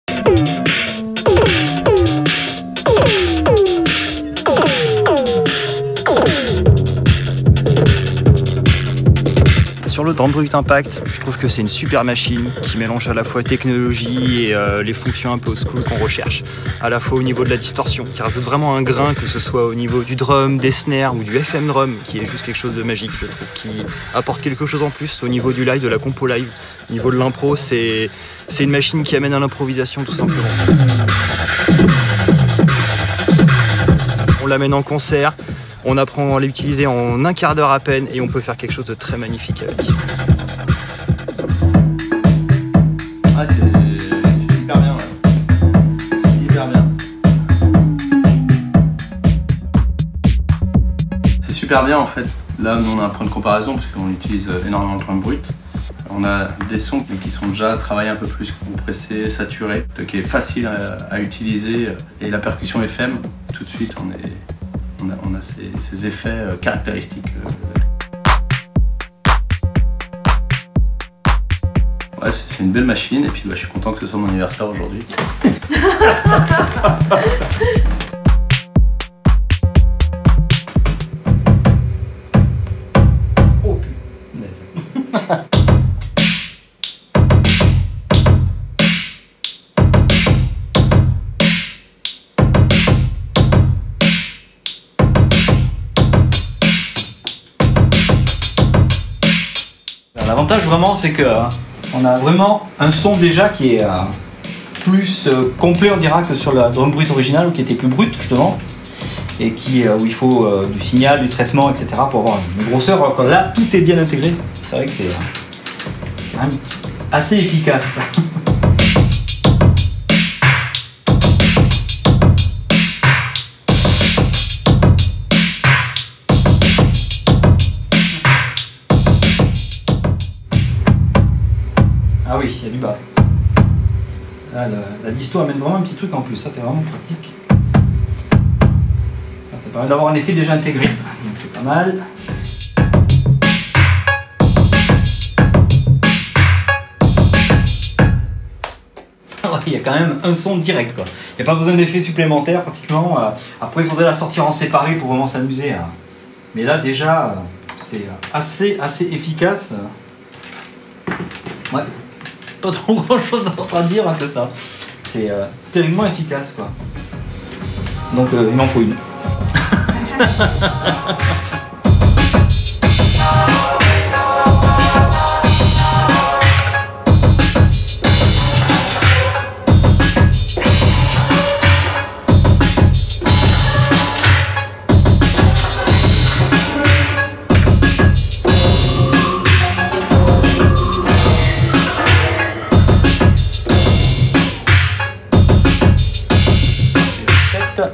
Arturia DRUMBRUTE IMPACT : réactions à chaud des artistes (vidéo de La Boite Noire)
Nous sommes allés à la rencontre de Proto Xide (Sound System les Ravaleurs de Façades), de Marylou In Time et de JMM (studio Alkemiae) pour découvrir leurs impressions à chaud sur le DRUMBRUTE IMPACT de chez Arturia.

Les boites à rythme Arturia sont devenues une référence en la matière avec le Drumbrute.

FICHE TECHNIQUE

Catégorie :
boîte à rythmes
Technologie :
analogique
Nbre d'instruments :
10
Échantillonneur :
Non
Nbre de voies :
10
Mode song :
Non
Mode looper :
Non
Nbre de pads :
8
Séquenceur :
Oui
Effets intégrés :
Oui
Polyphonie max :
10
Nbre de pattern :
64,00
Mode autonome :
Oui
Stockage :
sur ordinateur externe
Mémoires utilisateur :
64
Synchro :
clock, interne, Midi, USB
Sorties :
jack
Alimentation :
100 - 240 V AC - 50/60 Hz
Logiciels fournis :
Midi Control Center
Dimensions (mm) :
350x290x90
Poids (kg) :
1,84